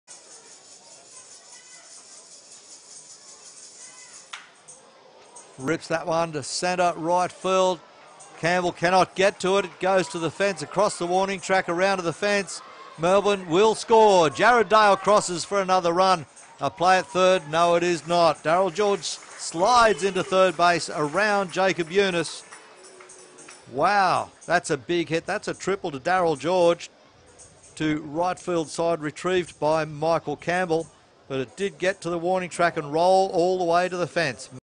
GEORGE TRIPLES HOME DALE | MEL @ SYD / G4
Darryl George gives the Aces their third run of the game, brining home Jarryd Dale on an RBI triple.